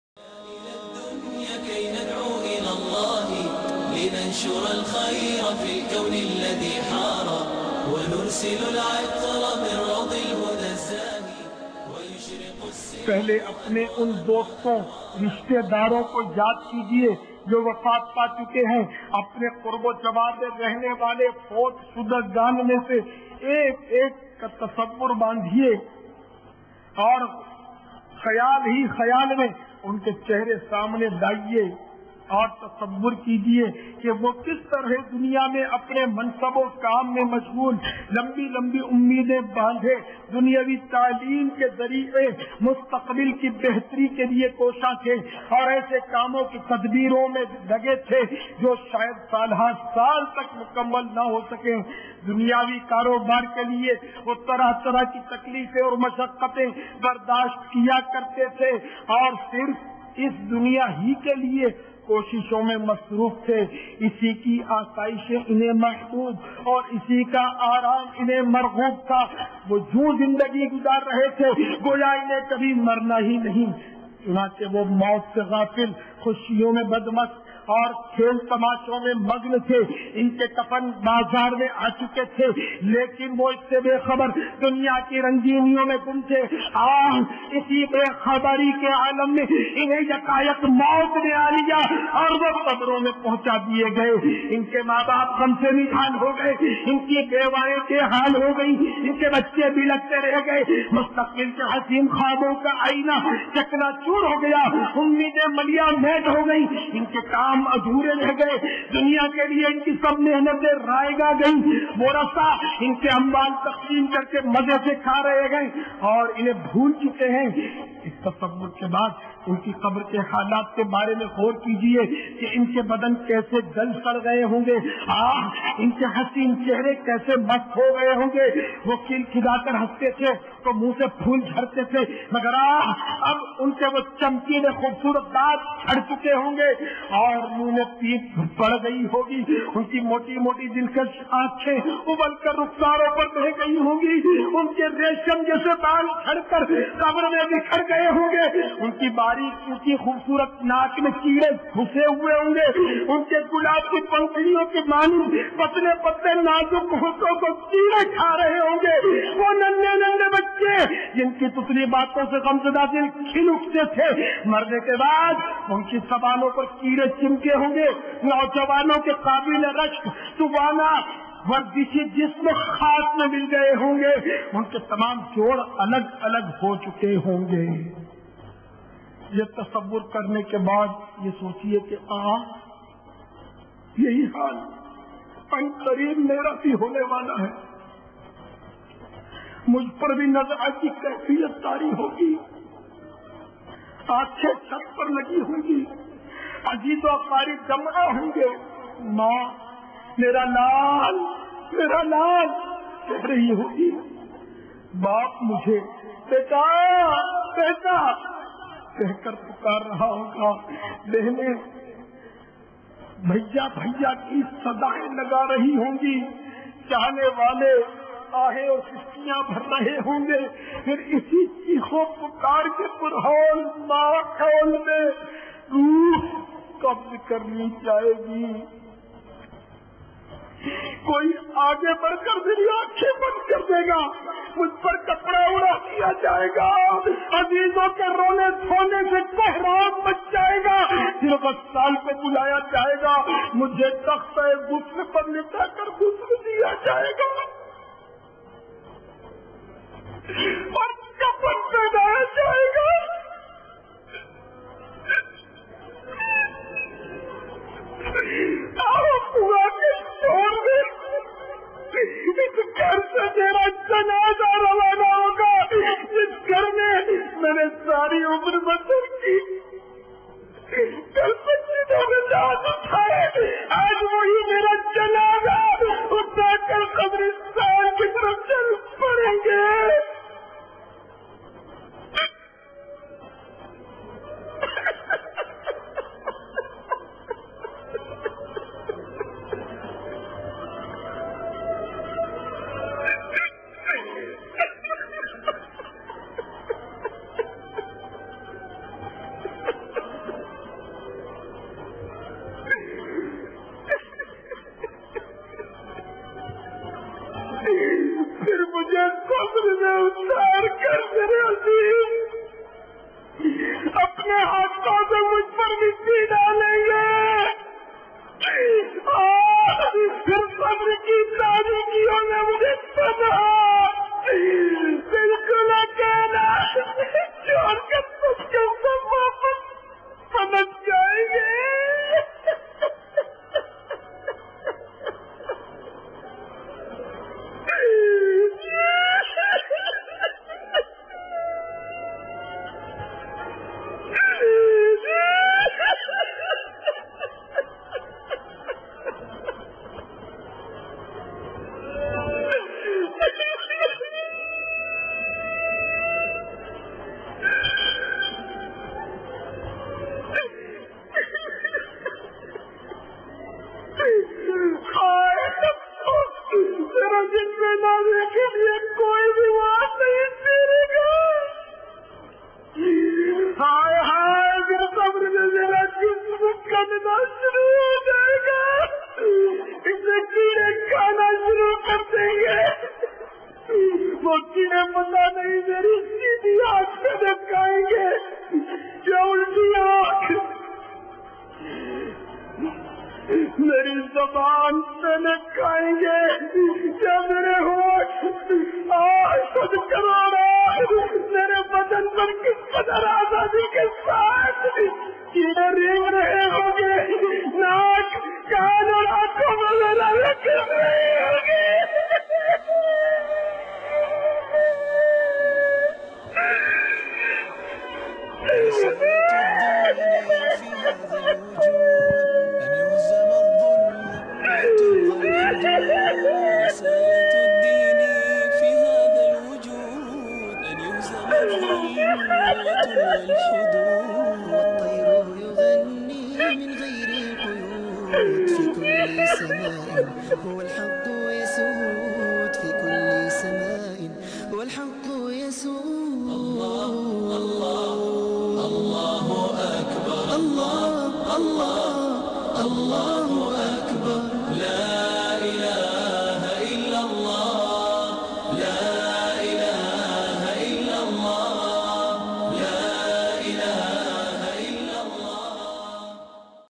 First Night In Grave Extremely Emotional Qabar Ki Pehli Raat
THE ISLAMIC LEARNING CHANNEL Provide Daily Basis Islamic Videos And Much Much More...

We reupload these videos because we love islam and we spread the islam all over the world.